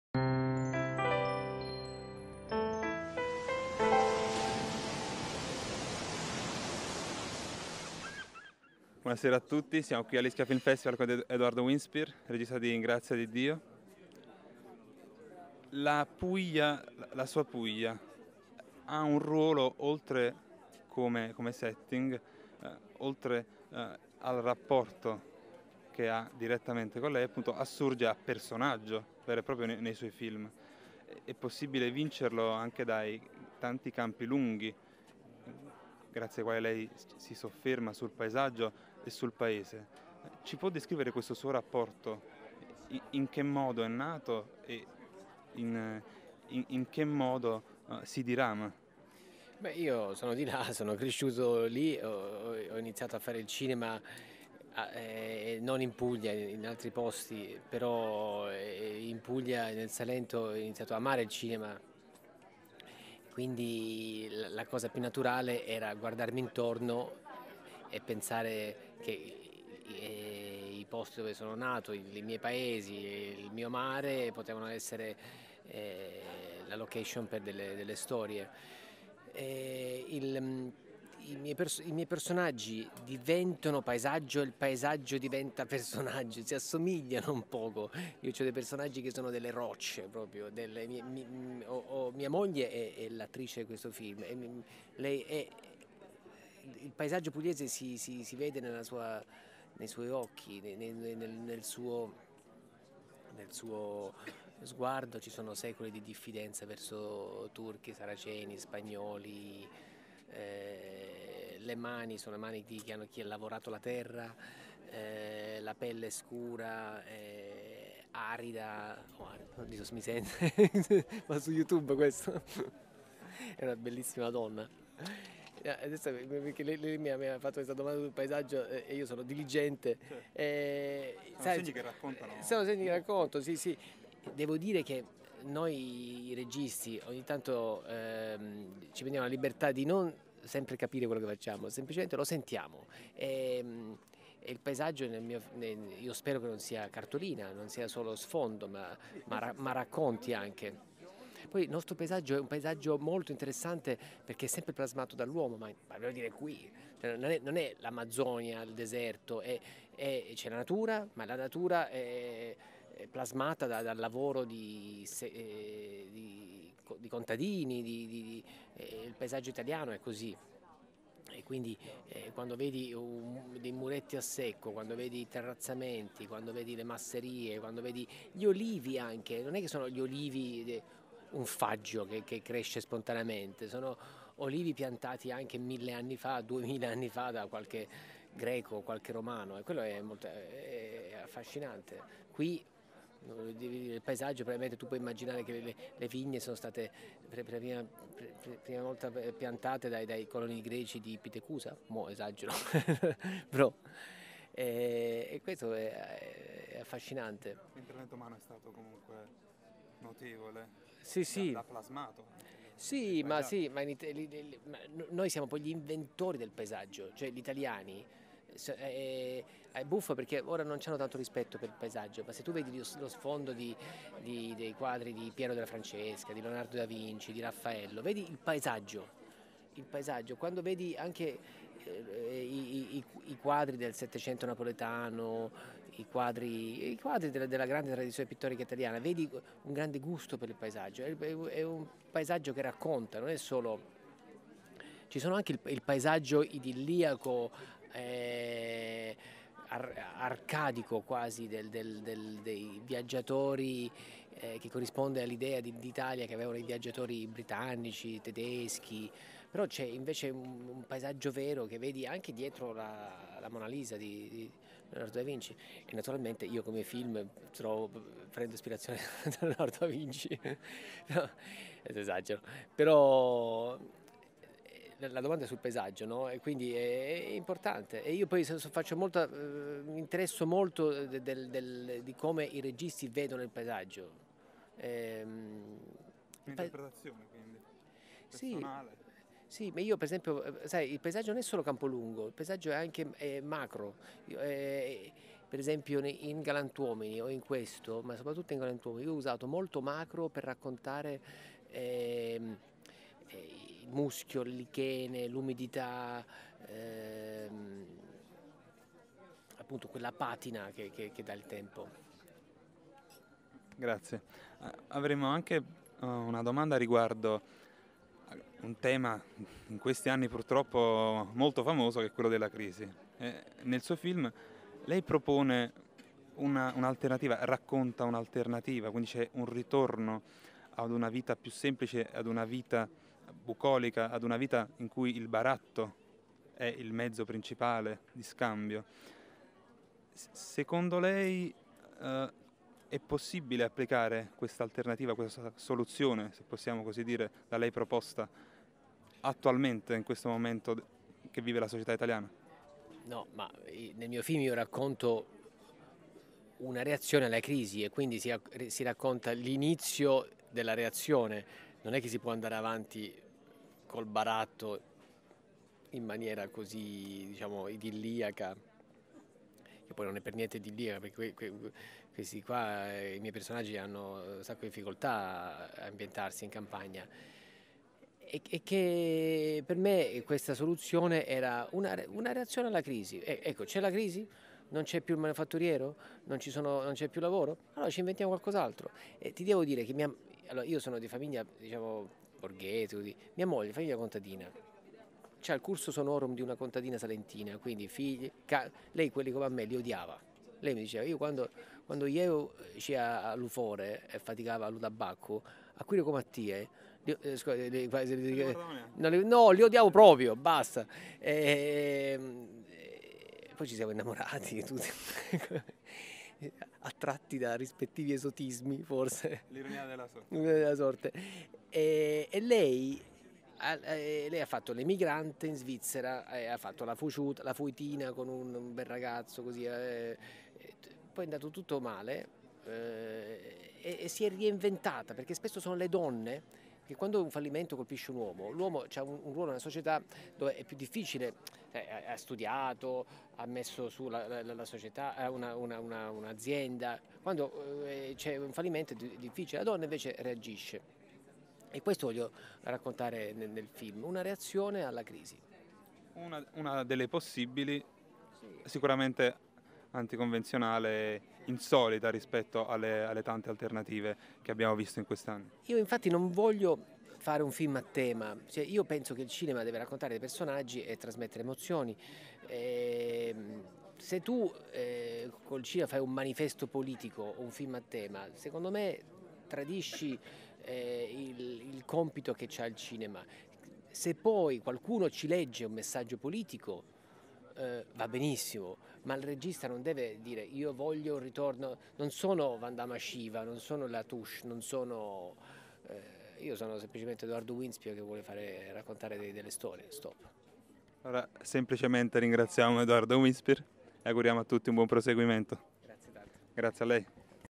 Edoardo Winspeare all'Ischia Film Festival 2014
Il regista Edoardo Winspeare, dopo i successi di Berlino, presenta all'Ischia Film Festival il suo ultimo successo "In Grazia di Dio". Intervista a cura di Last Scugnizzo.